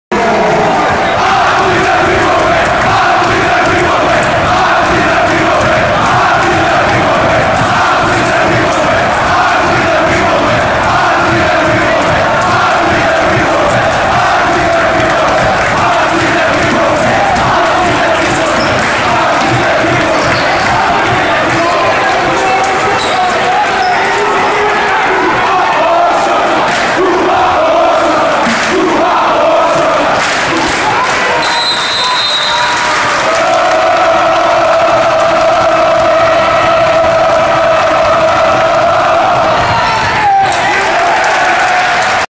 U of D Jesuit Cubs student section
The eccentric U of D Jesuit Cubs student section cheers on their team against rivals Brother Rice at home Friday night, Jan. 23, 2015. hssn (Tanya Moutzalias | MLive Detroit)